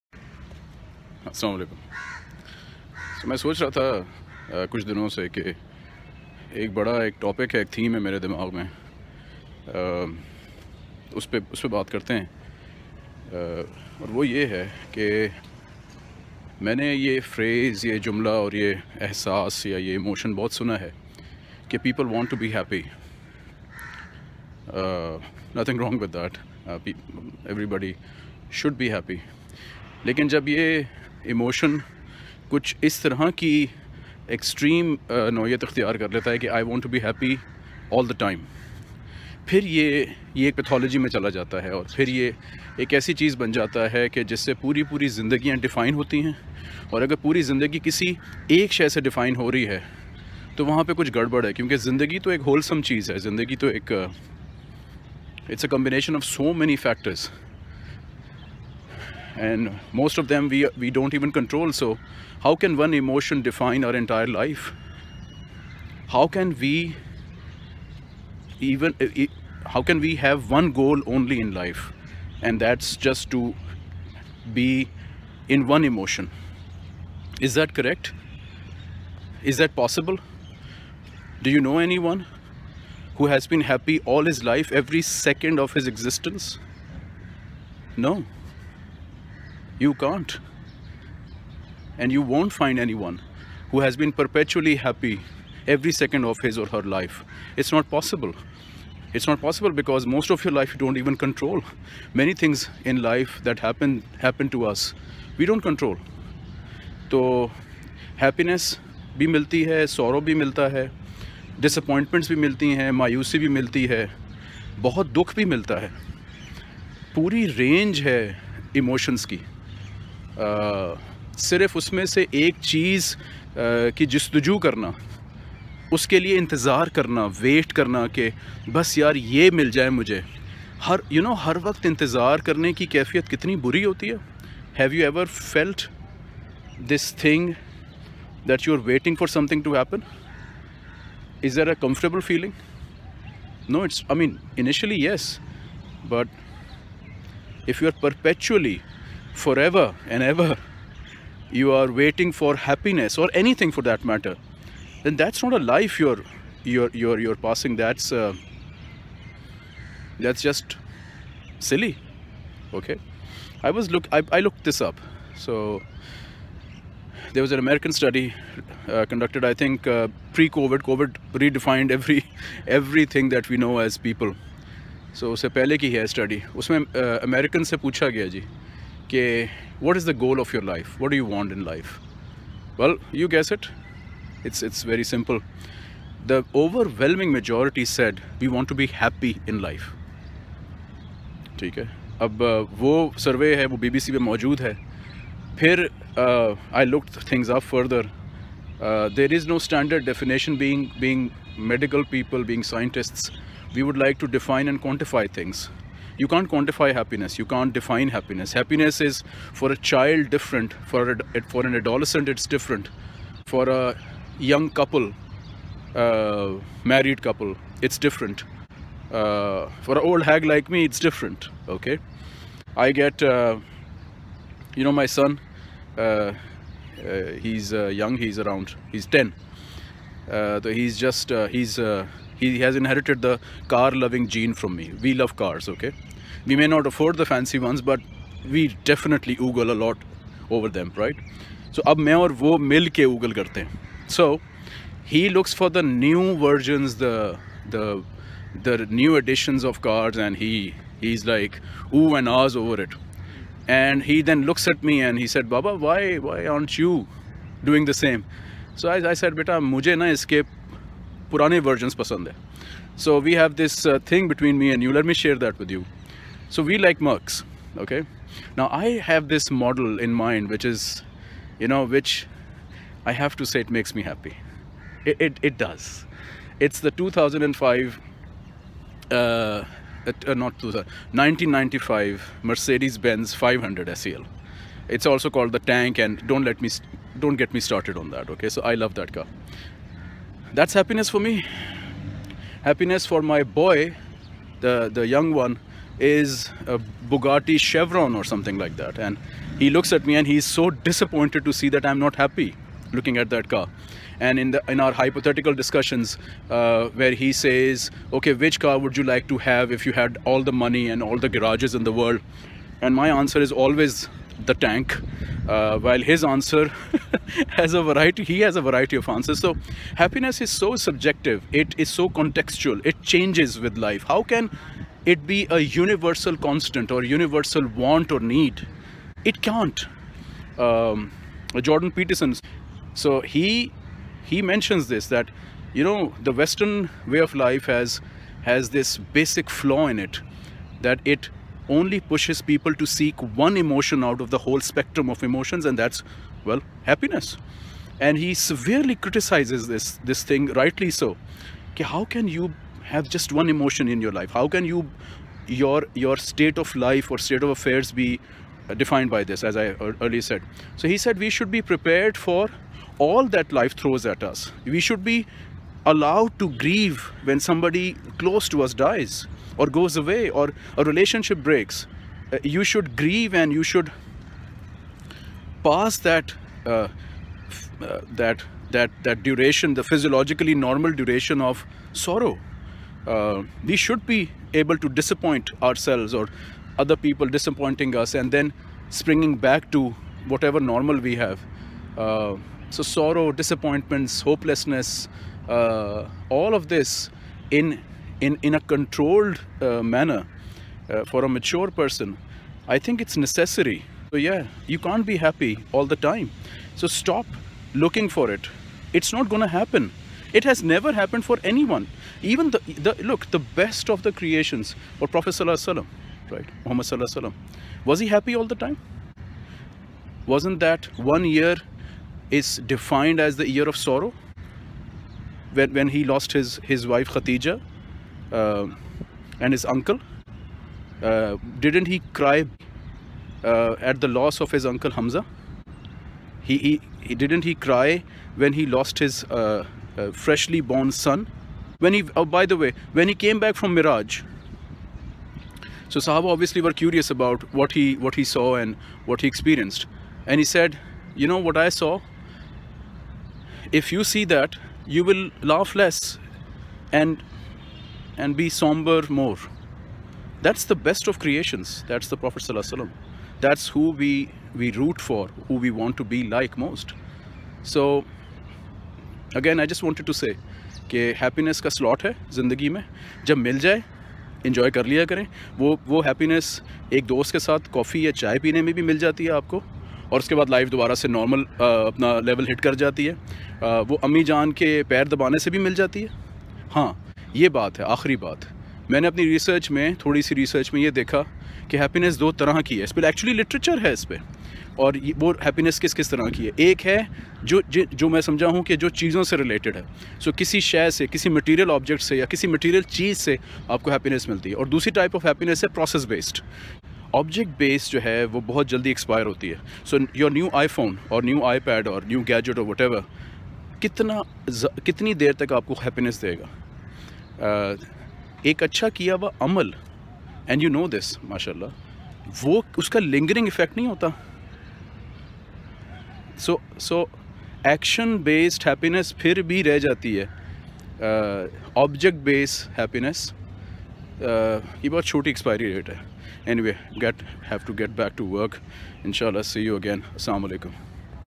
Life lessons: ...and they lived happily ever after.... REALLY!!
Happiness is one state of mind - one out of many. It is an emotion in a whole range of emotions. We should be able to handle ourselves in a variety of situations, not just one of them.